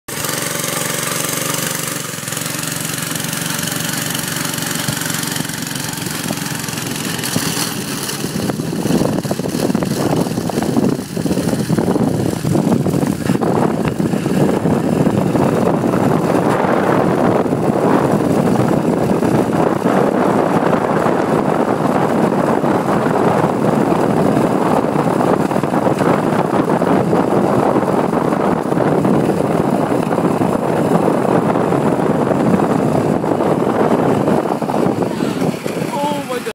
air bagged trike draggin
trycicle i built with a 6.5hp honda clone engine and air ride suspension in the rear.will post full video of it not moving,and yes the axle broke while i was draggin but i fixed it.